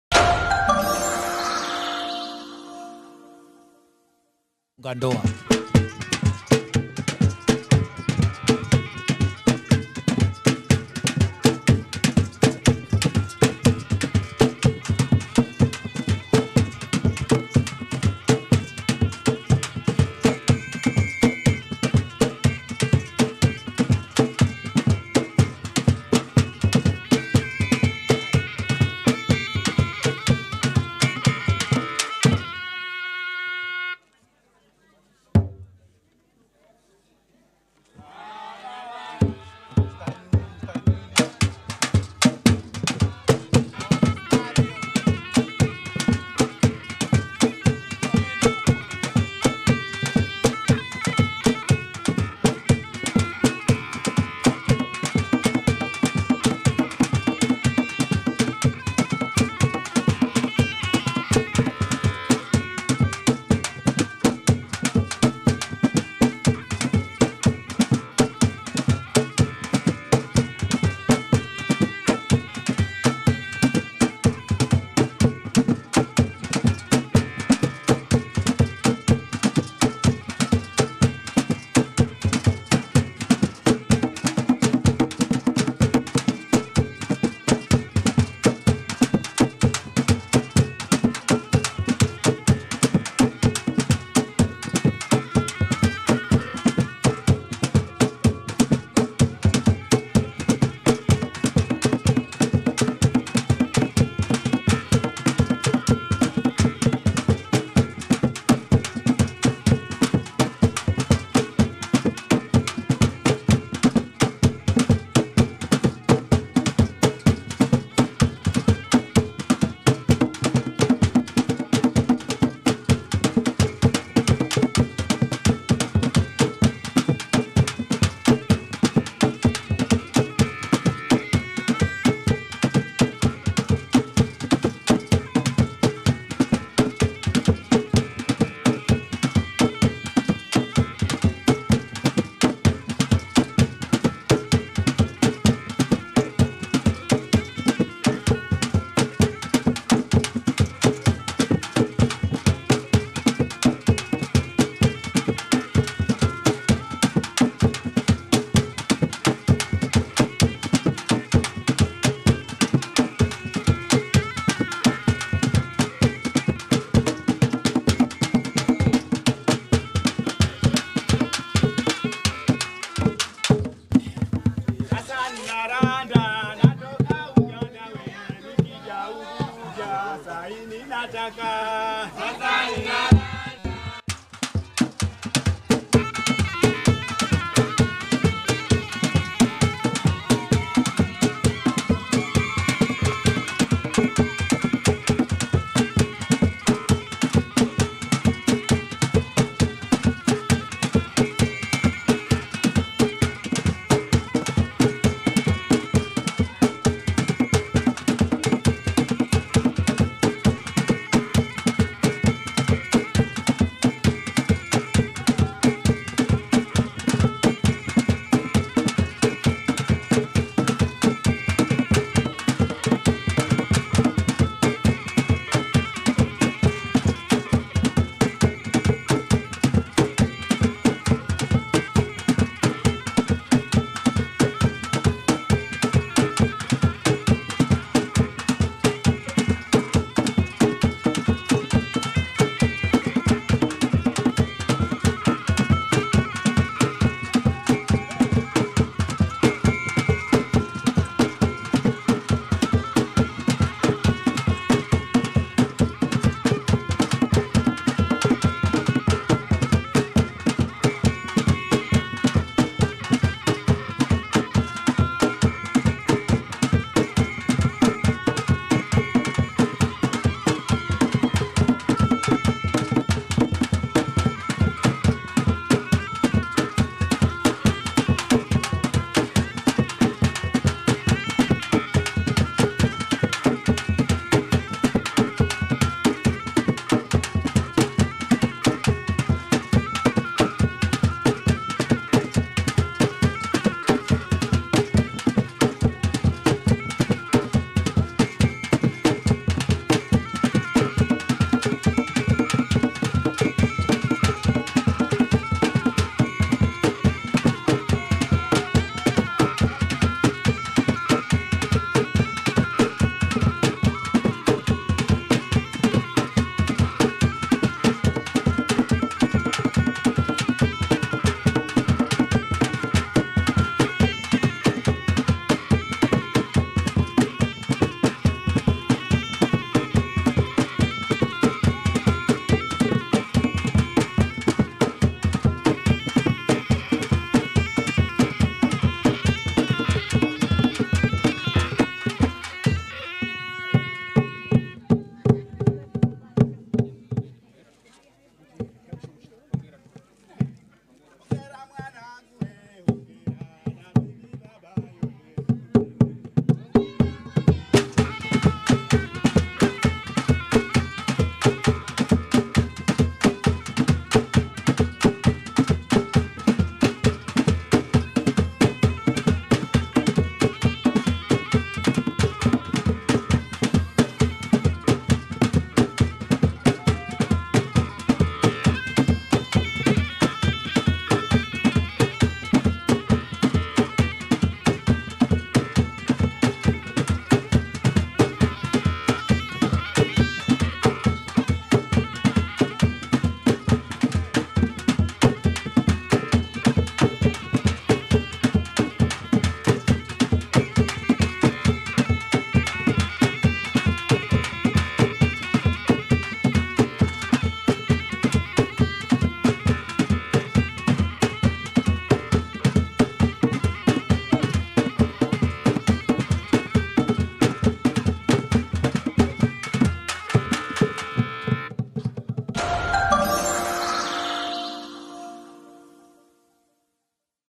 NGOMA YA MDARANGA
Ngoma hii inachezwa Donge